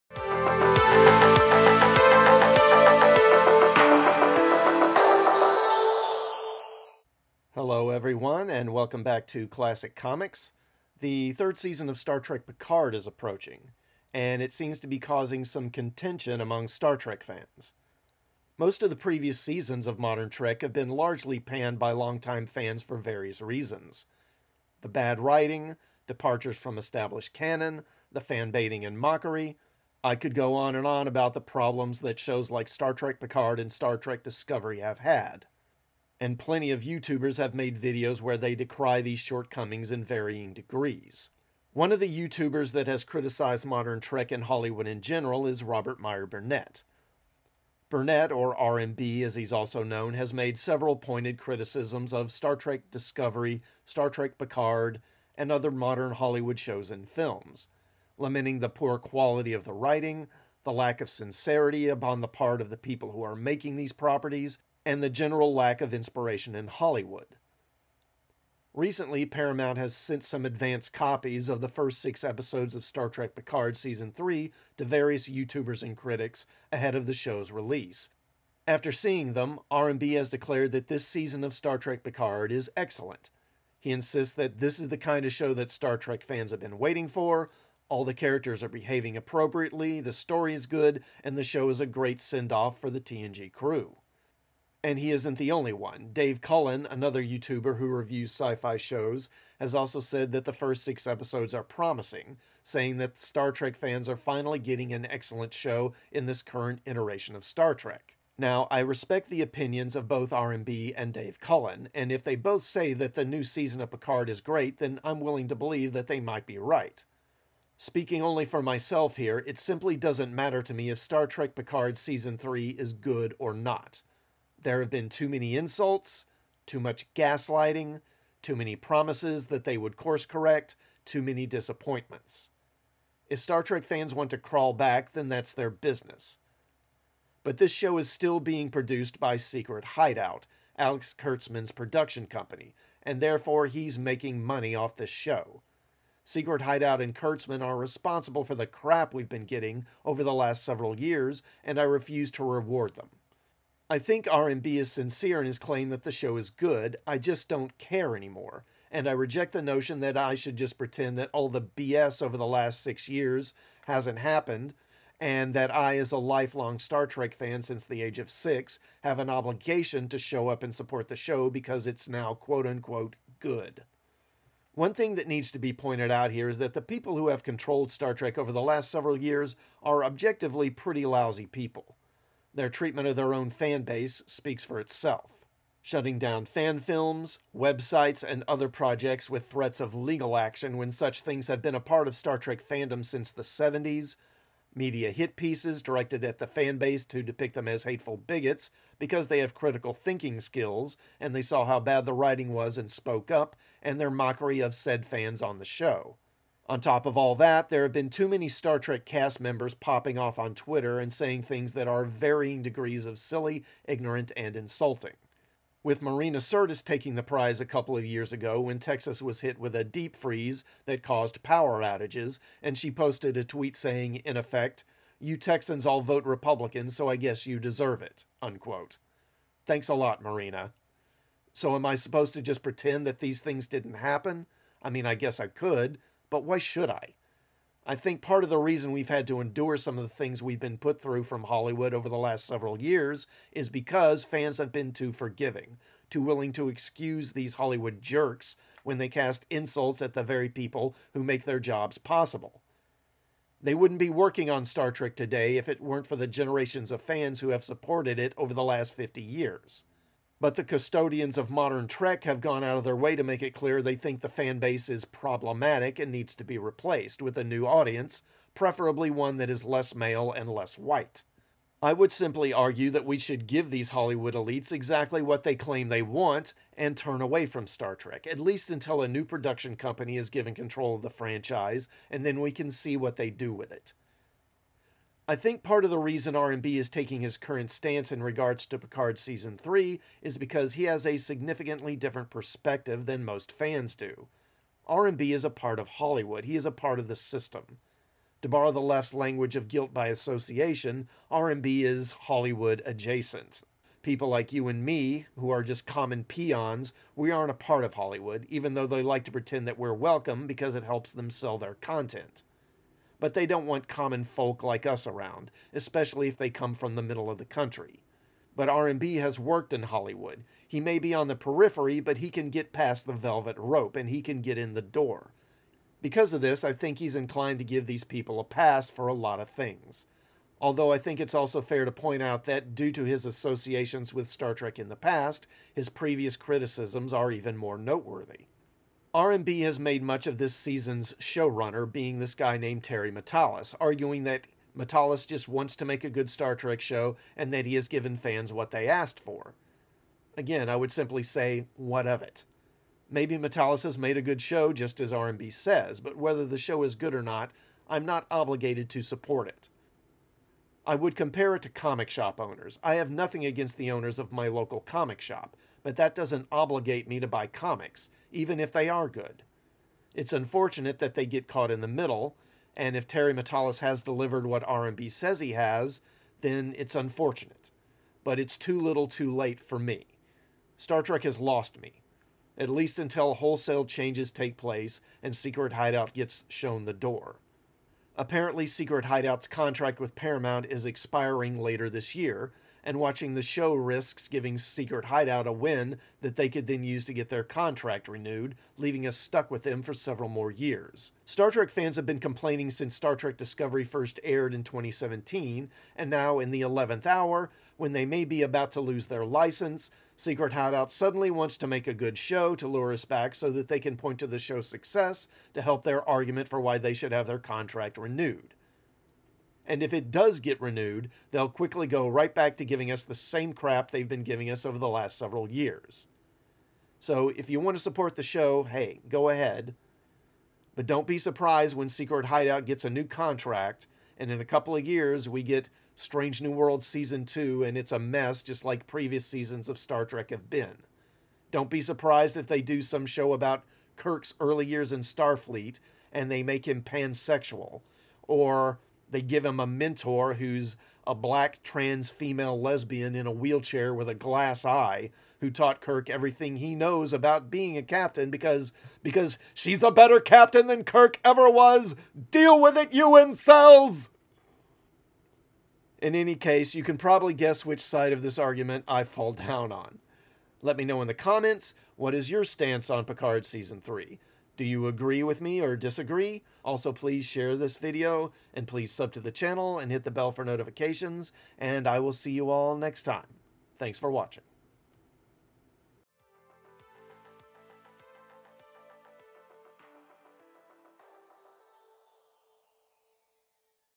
TOO LITTLE TOO LATE: WHY IT DOESN'T MATTER IF SEASON 3 OF STAR TREK PICARD IS GOOD OR NOT
The third season of Star Trek Picard is coming and some are saying that the new season is actually pretty good, but I still won't be watching no matter how good it is.